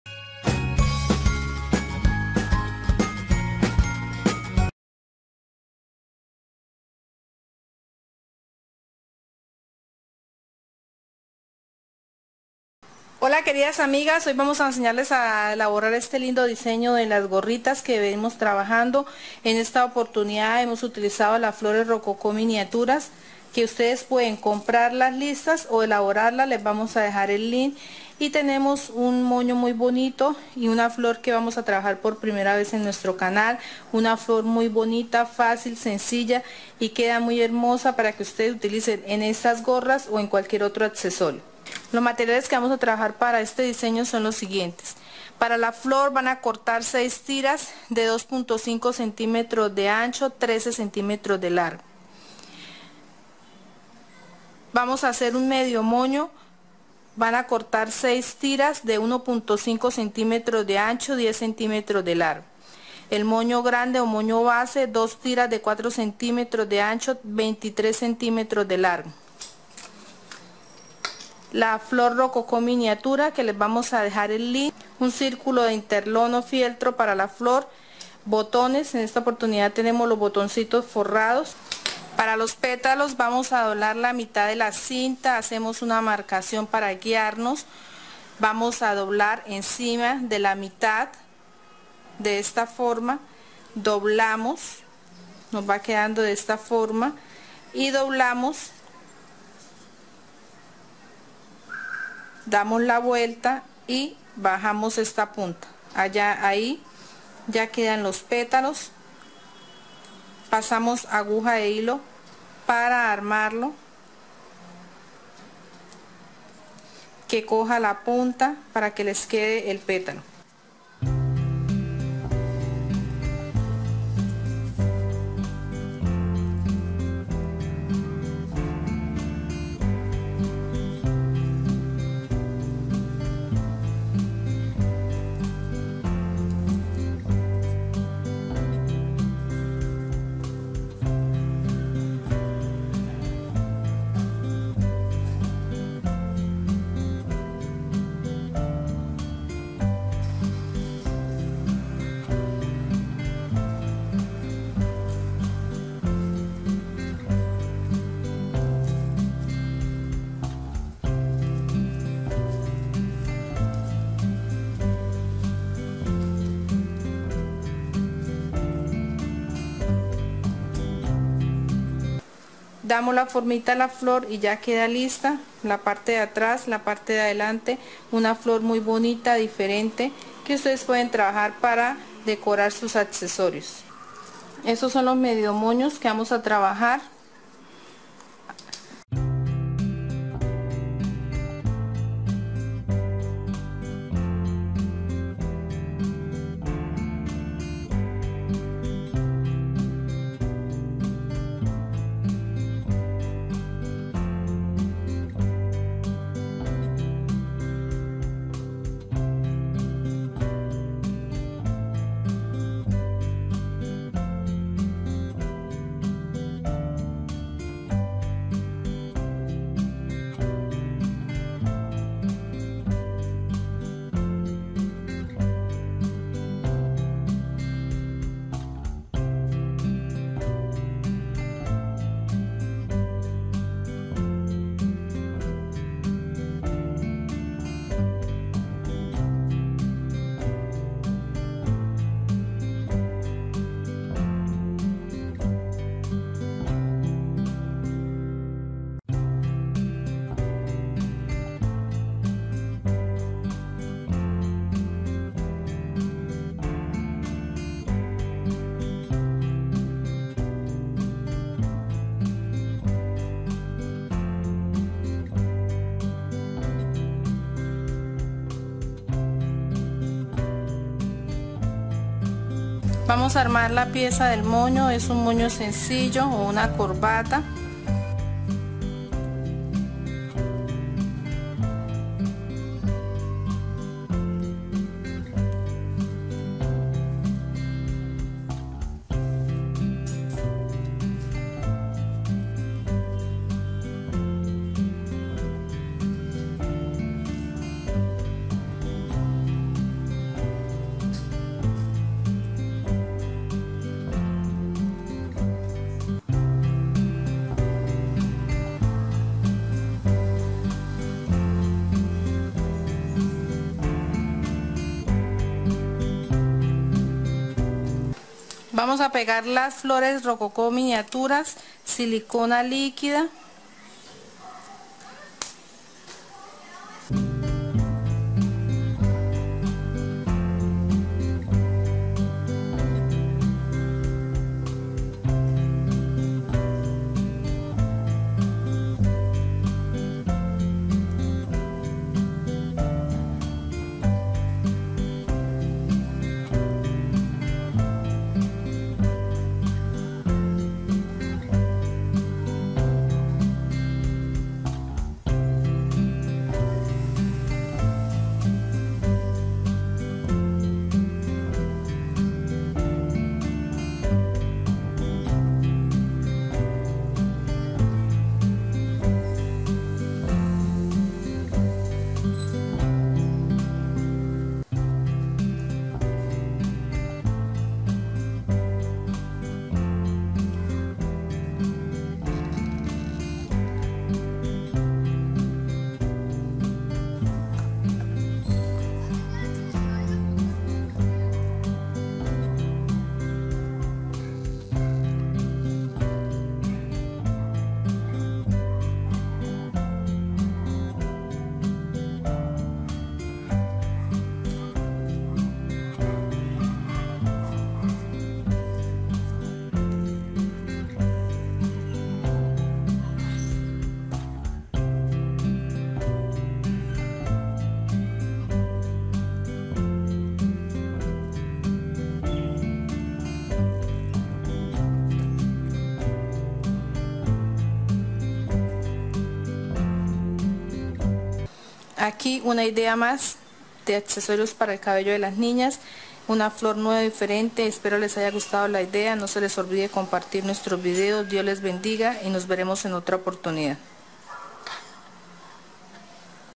COMO HACER MOÑOS Y FLORES para Gorras de Niñas/How to Make Ties/ Accesorios para el cabello
ideas de accesorios para el cabello
para regalar en fiestas